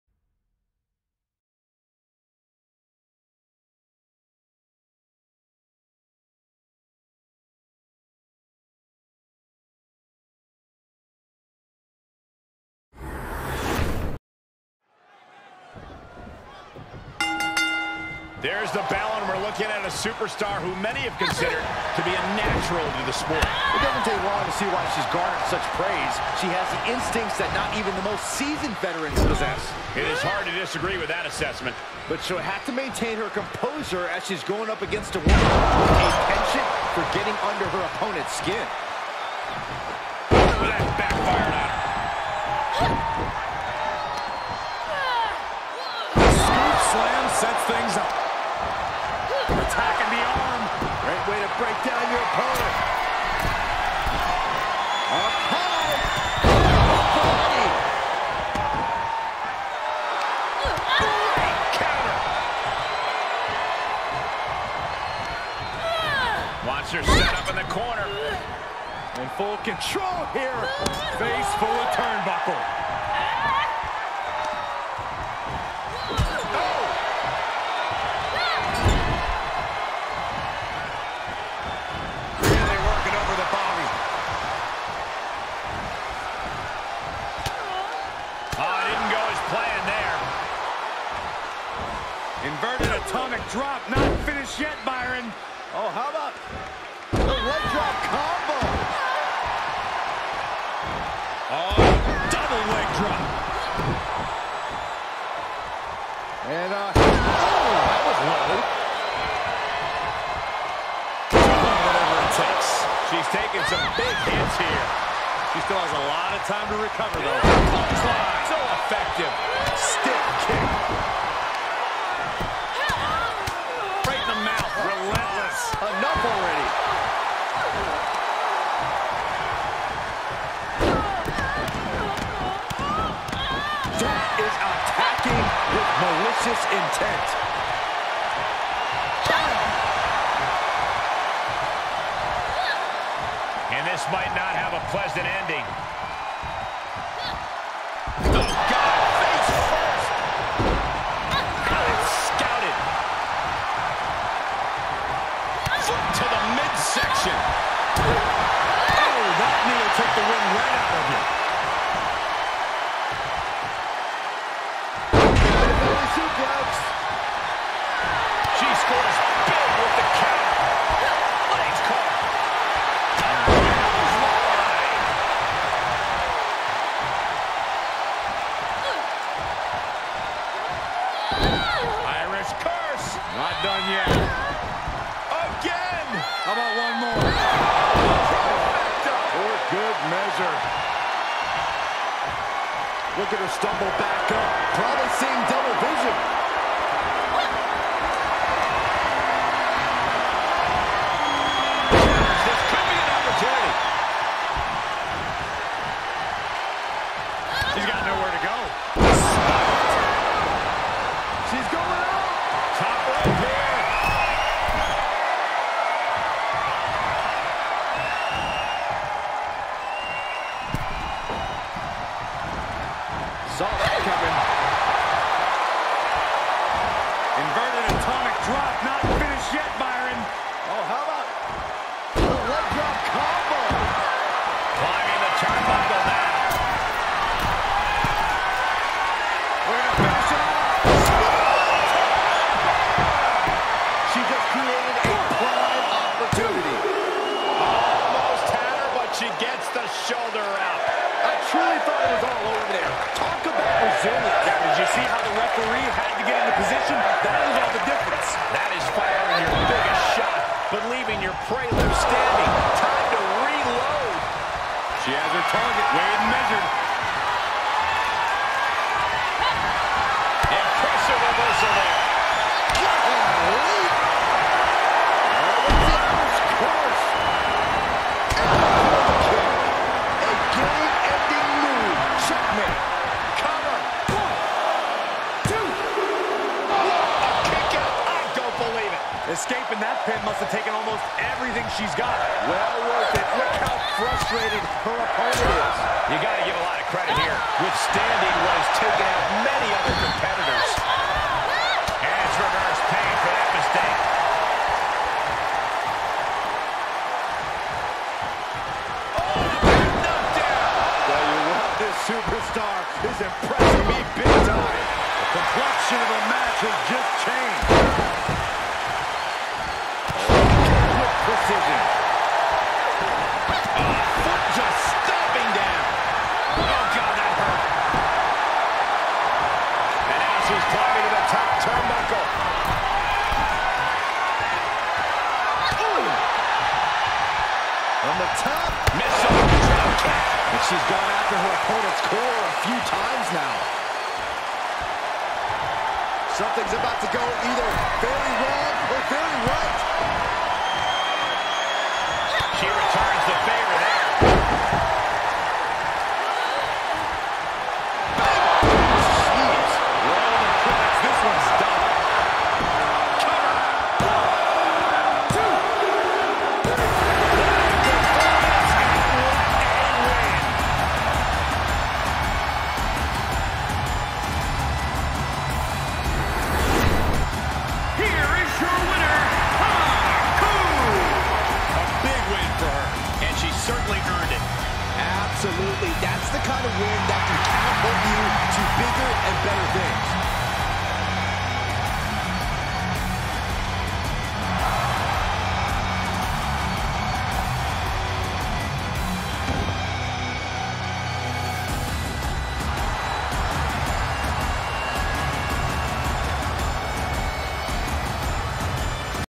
Kizuna Ai vs Haru Okumura
Since i don't have Kizuna Ai potrait yet lets use the Default setting. The 5th Phantom Thieves Lady debut match, DAMN! What a finish!